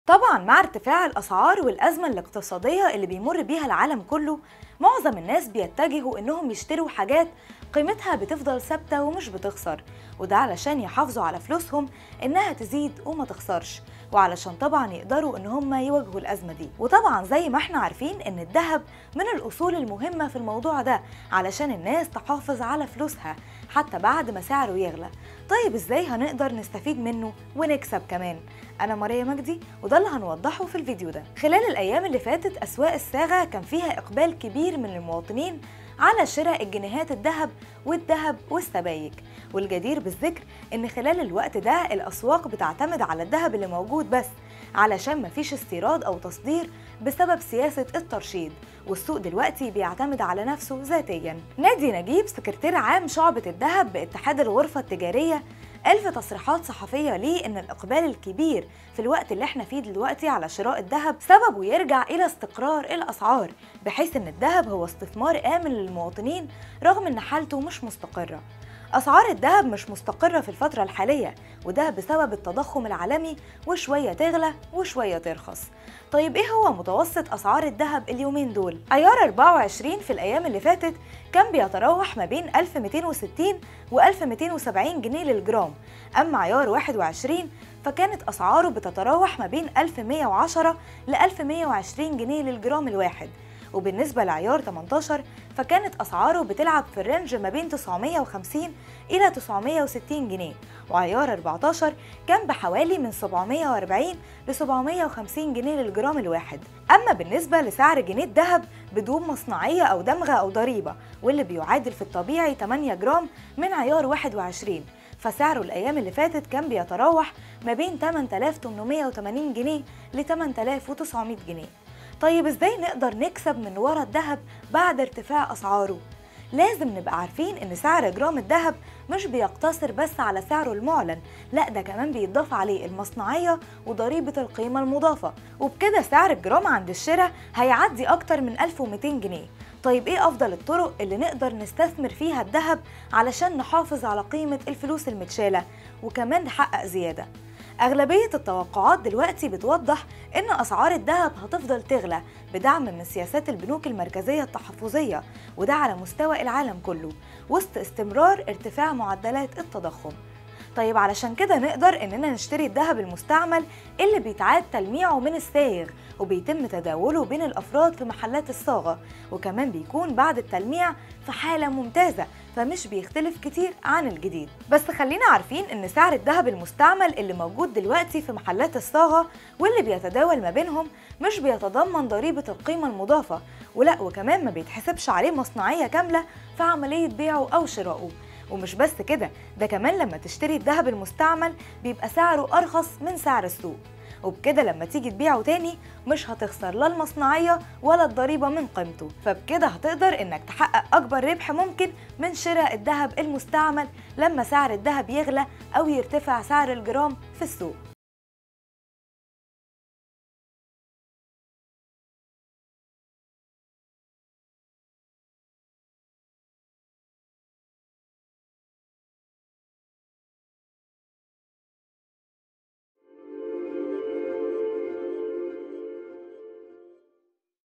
إقبال كبير على سبايك الدهب والكل رايح يشترى.. إزاى تكسب زيهم حتى لو هتشتريه بالغالى؟
طبعا مع ارتفاع الاسعار والازمة الاقتصادية  اللي العالم كله بيمر بيها دلوقت، معظم الناس بيتجهو إنهم يشتروا حاجات قيمتها بتفضل ثابتة ومش بتخسر، وده عشان يحافظو على فلوسهم إنها تزيد ومتخسرش، وعشان يقدرو يواجهو الازمة دي.

وطبعا زي ما احنا عارفين ان الدهب من الاصول المهمة في الموضوع ده  للناس عشان تحافظ علي فلوسها، حتى بعد ما سعره غلي، طب ازاي هنقدر نستفيد منه كدة ونكسب منه؟ ده اللي هنوضح تفاصيله في التقرير اللي جاي.